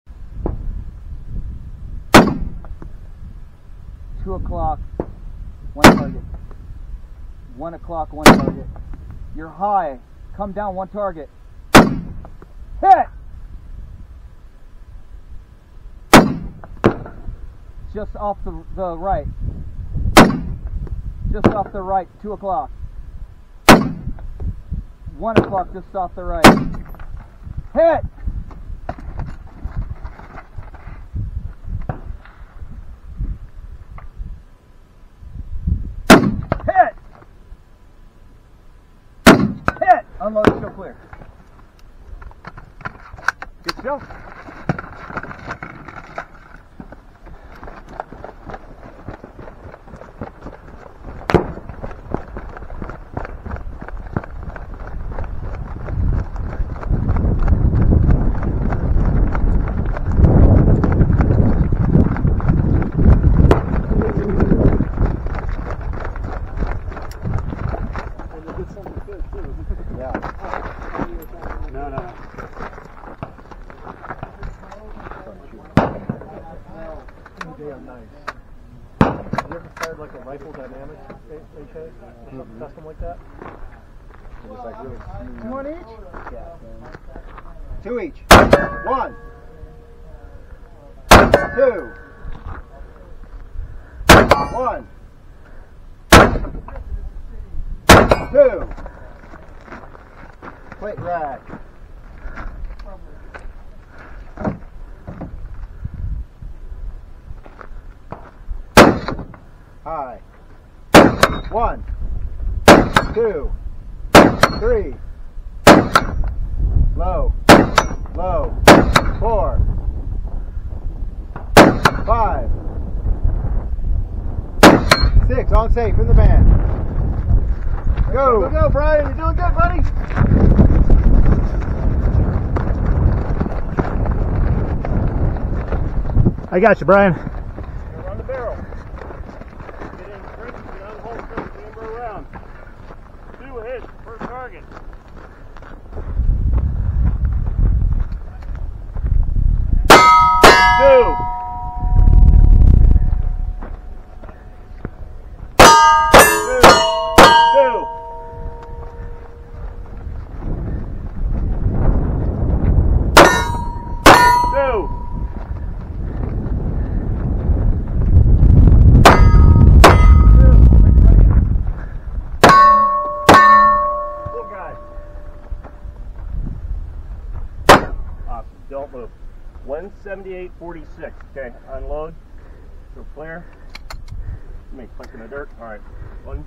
High Desert Brutality 2023 Stage 5 - Operation Wolf
My run at High Desert Brutality 2023 of Stage 5  - Operation Wolf

Final time on this stage: 178.46s
55th overall
29th in Partizan division